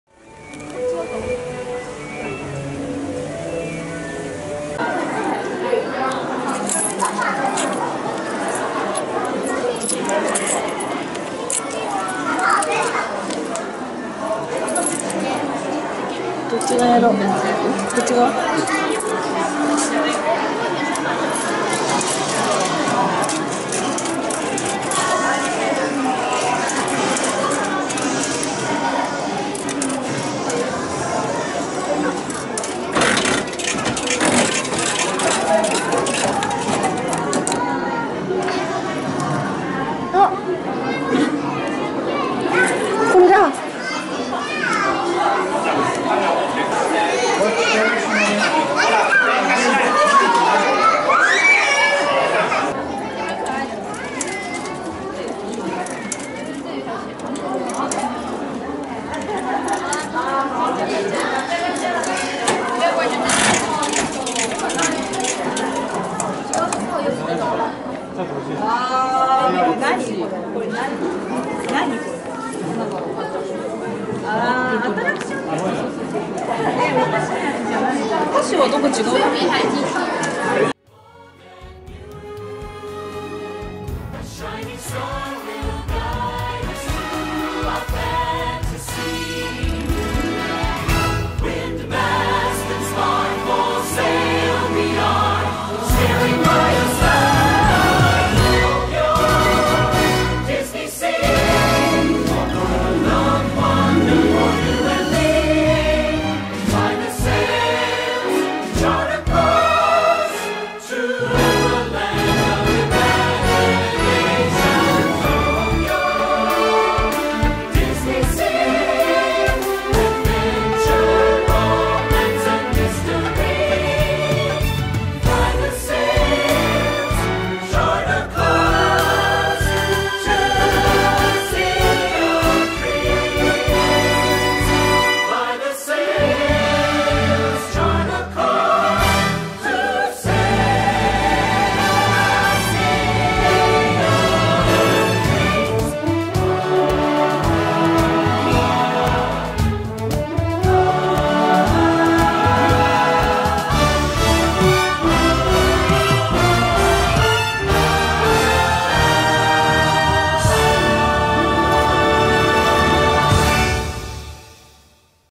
「マーメイドラグーン」エリア屋内ショップに設置のガチャガチャをやってみよう！
ディズニーシーにはガチャガチャが2箇所のみ設置されています。
マーメイドラグーンエリア屋内ショップにあるガチャガチャをご紹介。
カプセルに入れたまま部屋に飾ってみてはいかがでしょうか。

DisneySea has only two clattering locations.
Introducing capsule toys in indoor shops in the Mermaid Lagoon area.
Why don't you decorate your room with it in a capsule